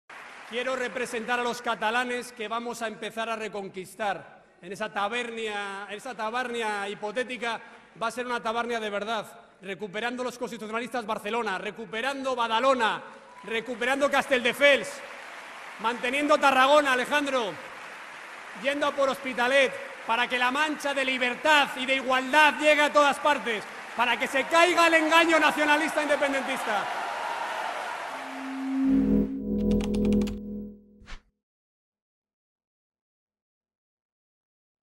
Casado: "Esa Tabarnia hipotética va a ser una Tabarnia de verdad"
Apóyanos.